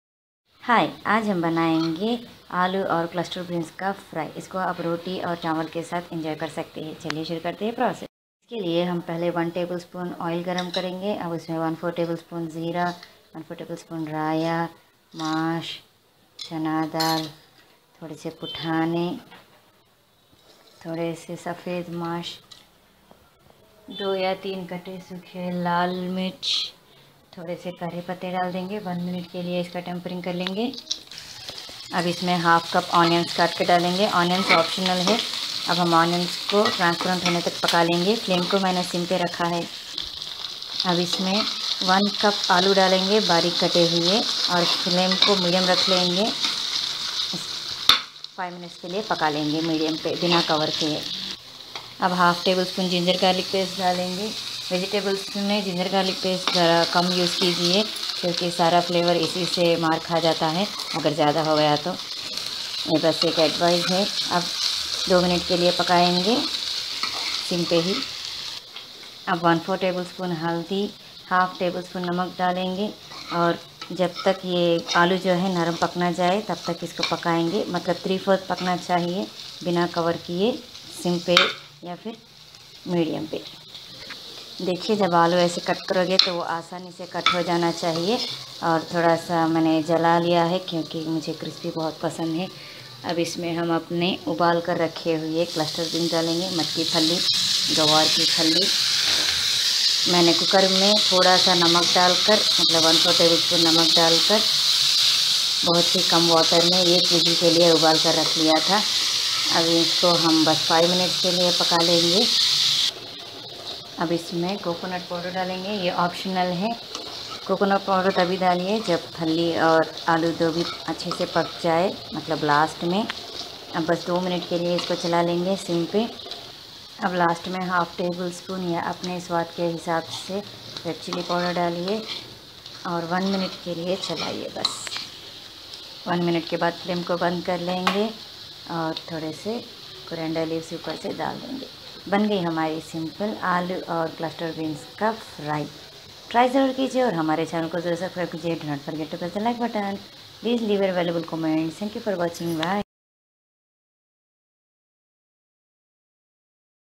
Guvar Phali aur Aloo ki Sabzi || Cluster beans and Potatoes fry
INGREDIENTS:

Bengal Gram - 1/4 tsp
Roasted Chana Dal - 1/4 tsp
Dry Red Chilli - 2 or 3
Potatoes - 1 Cup
Cluster Beans (Boiled) - 1 Cup
Coconut Powder - 1 tsp